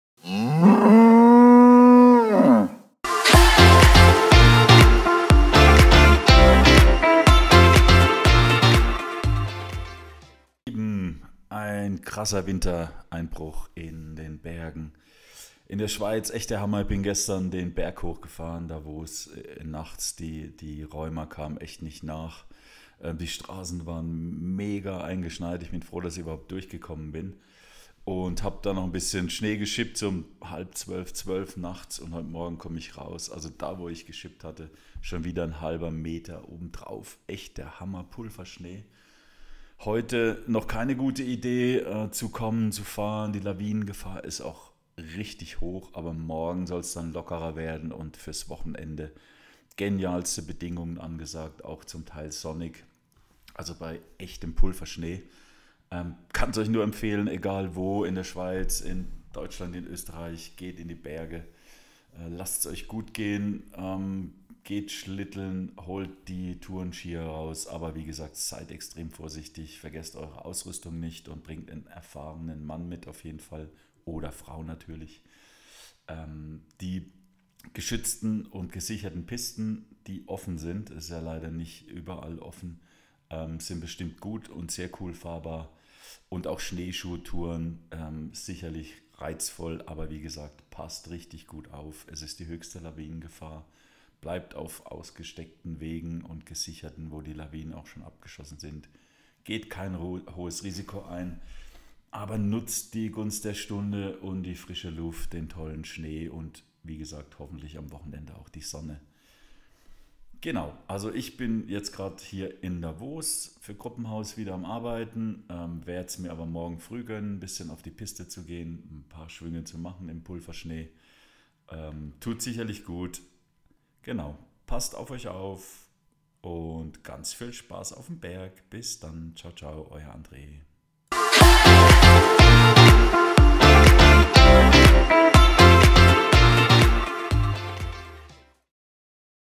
Wintereinbruch in den Bergen
Kommt in die Berge. Total krasse geniale Bedingungen. 1m Neuschnee